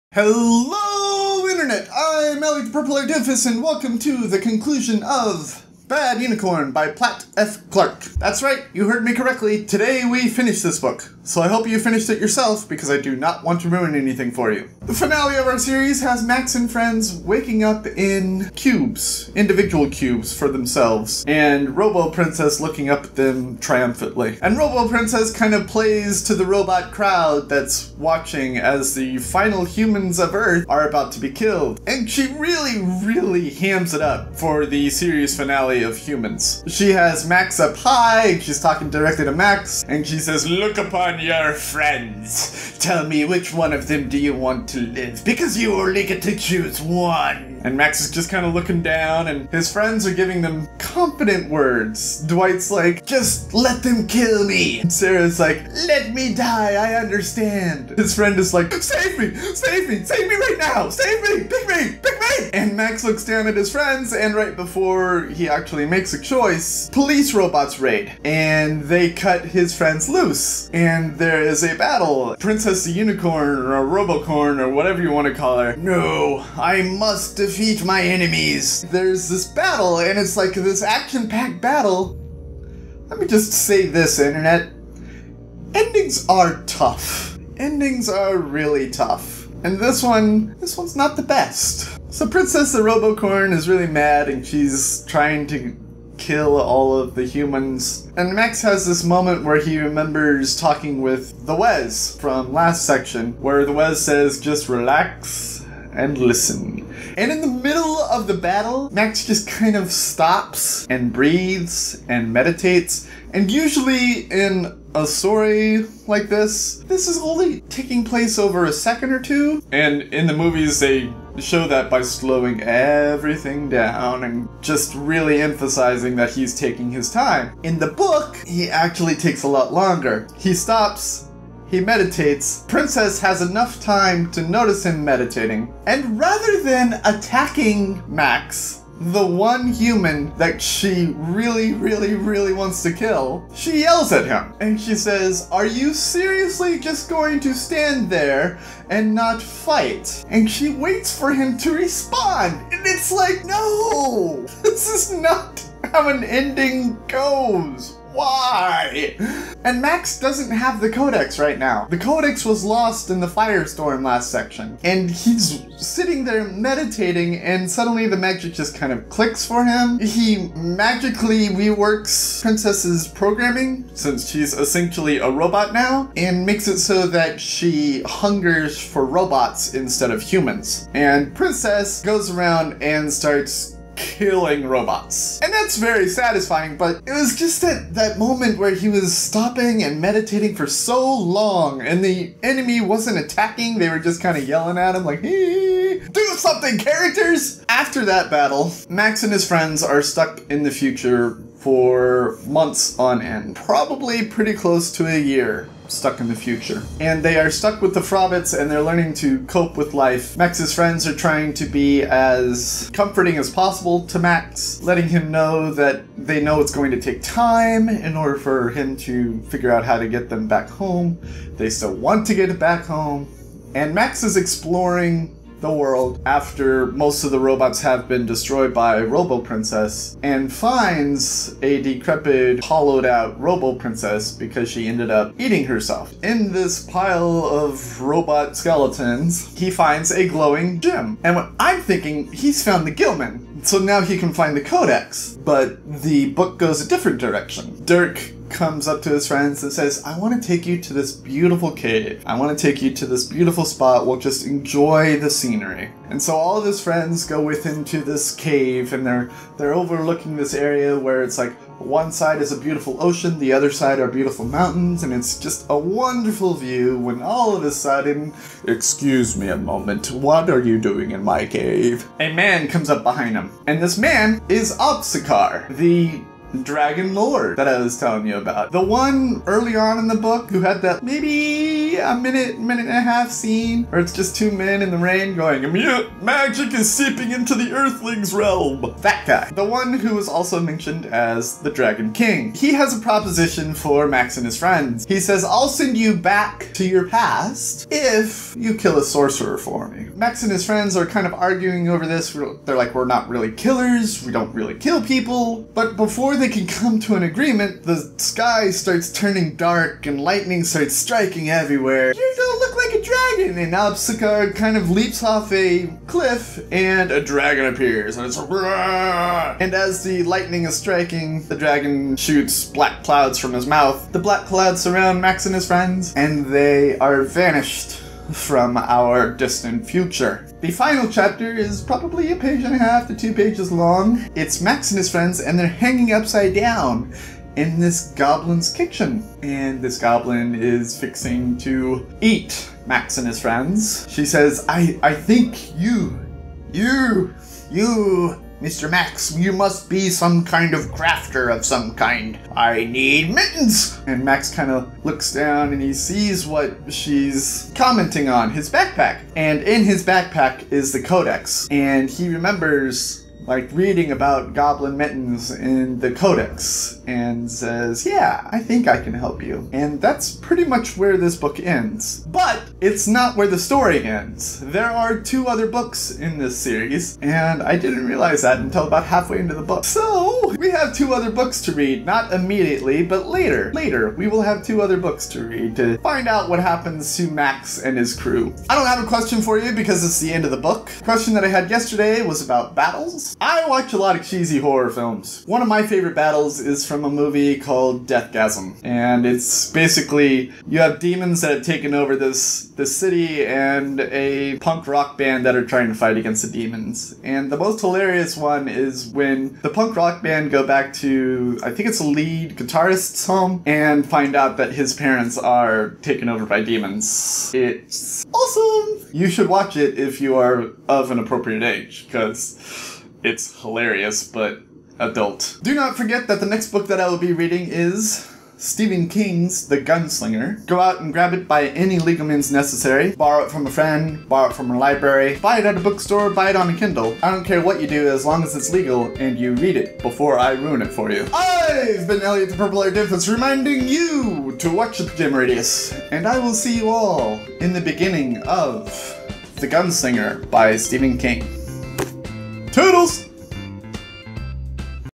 Finale BAD UNICORN 31 33
Today, we finish Bad Unicorn by Platte F. Clark